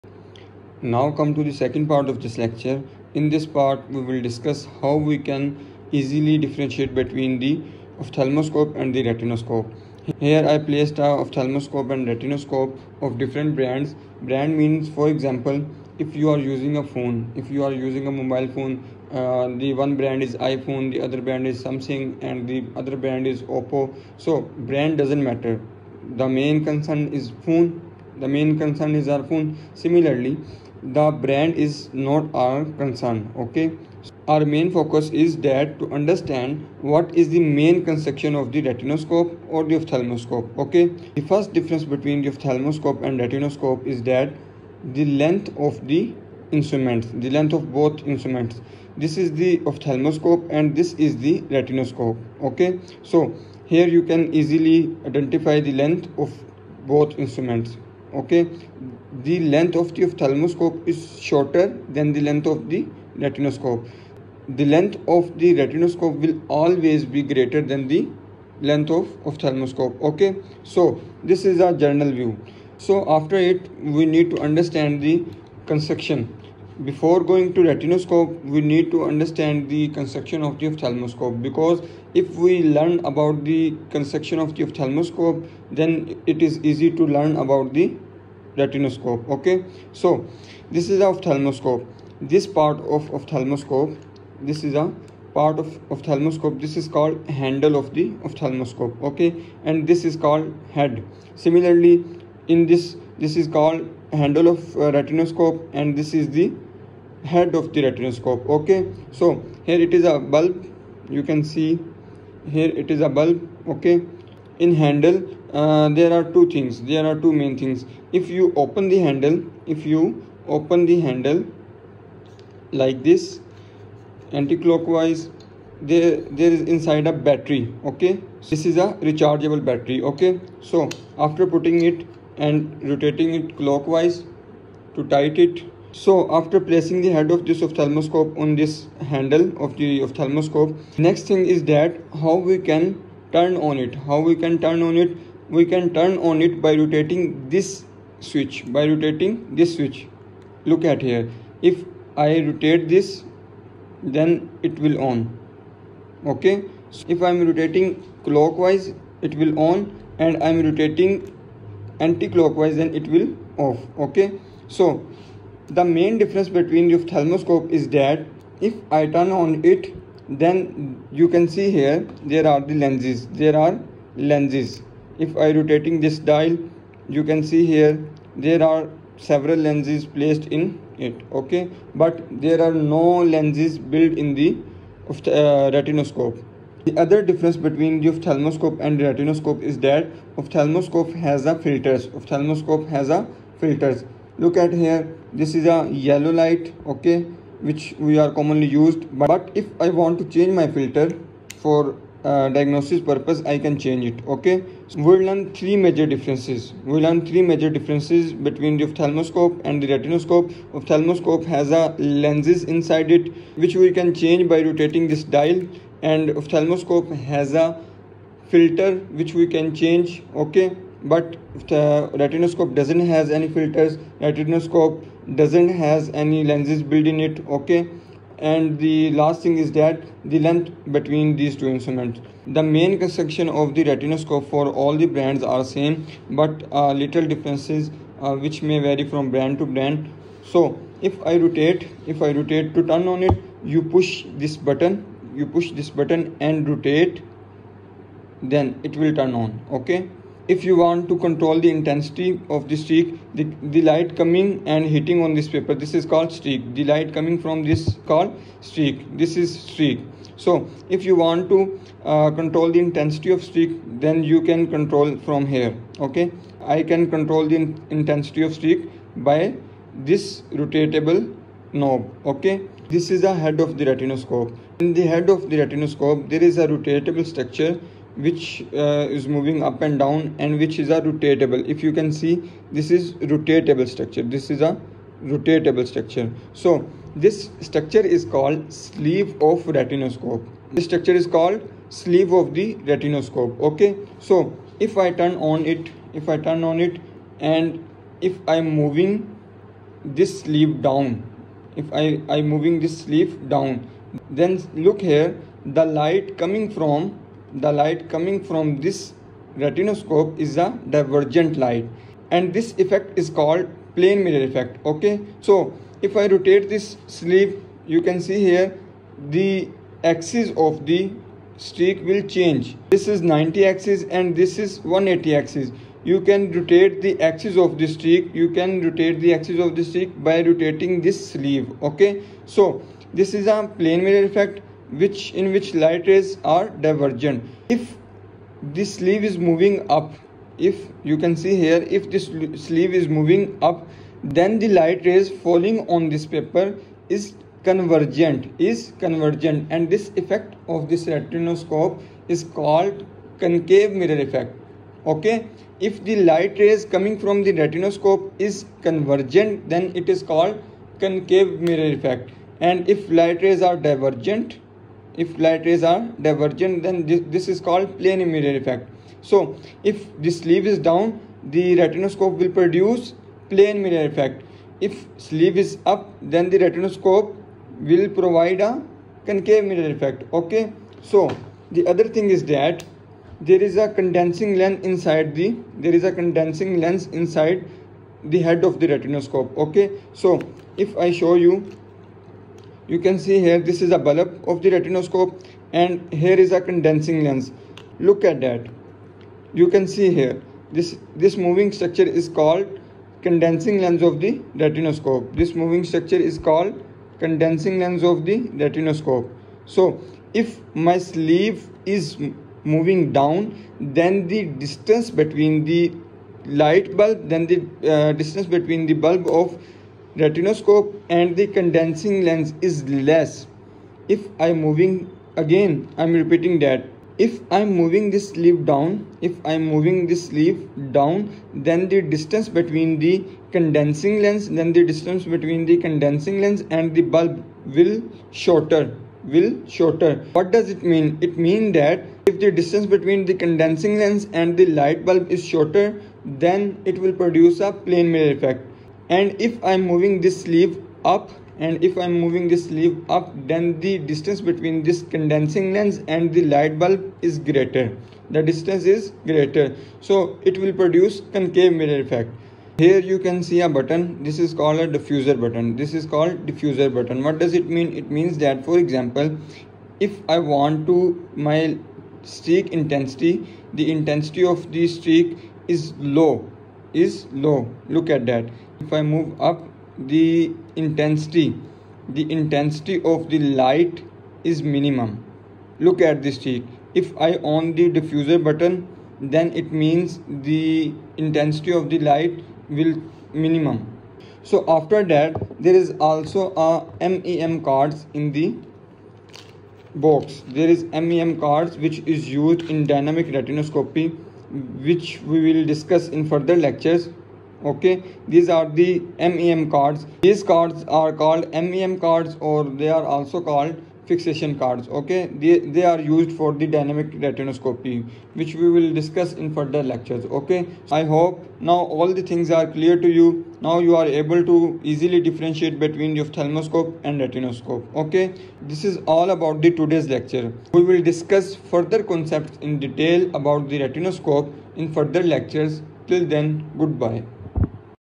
Ophthalmoscope vs Retinoscope | Difference & Construction Explained for Optometry Students/English
Confused between an ophthalmoscope and a retinoscope? You're not alone!
In this lecture, I clearly explain the difference between these two important eye examination tools and also break down their construction and parts in a way every optometry student can understand.

🎯 What you’ll learn in this video:

What is an ophthalmoscope vs. what is a retinoscope

Key differences in their function, use, and design

Internal construction and components of both instruments

Clinical use and significance in eye care practice

Easy-to-remember tips for students

This video is perfect for:

BS Optometry & Vision Science students

Ophthalmic assistants and refractionists

Eye care learners preparing for practical exams